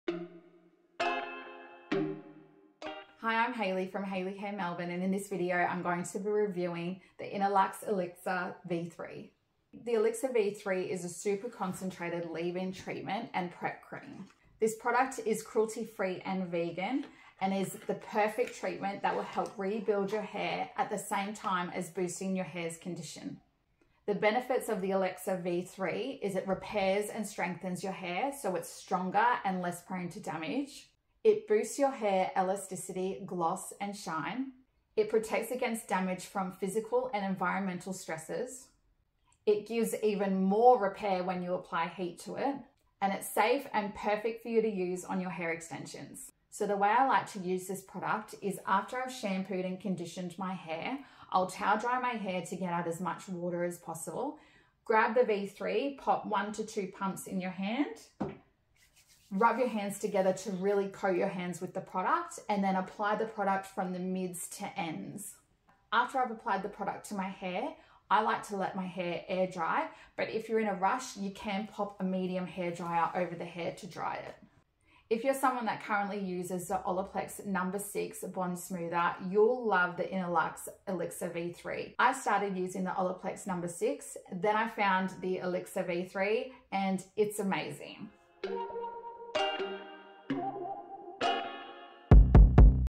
Innoluxe Elixir V3 Review (Better than Olaplex No.6)
If you currently use Olaplex No.6 Bond Smoother then you'll LOVE V3!
This product is perfect for your natural hair and hair extensions.

- Builds anti-break strength
- Enhances elasticity, gloss and shine
- Protects against physical and environmental stresses
- Includes heat-activated repair
- Cruelty free and vegan.